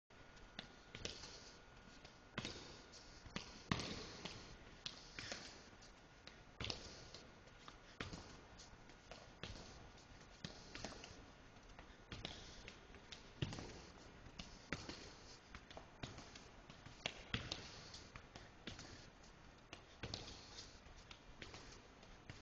SOLOSOCCER 2A: BALLWORK STUDENT
In SOLOSOCCER 2A we see a balltouch exercise that players use to learn at home.